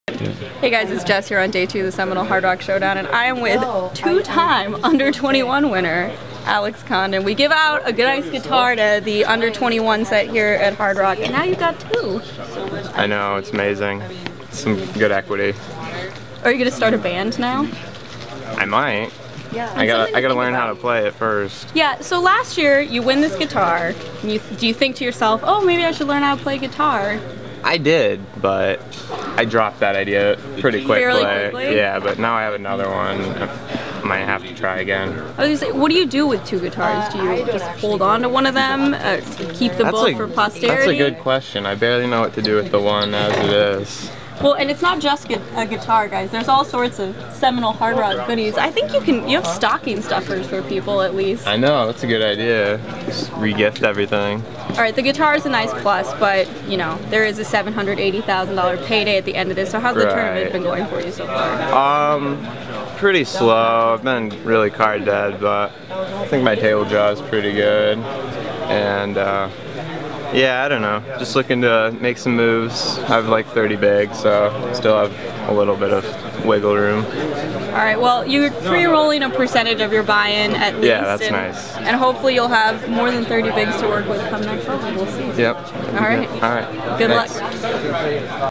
Last Under 21 Year Old Standing - Alex Condon
Jessica Welman interviews Alex Condon after he won the Last Under 21 Year Old Standing prize for the second year in a row at the Seminole Hard Rock Showdown.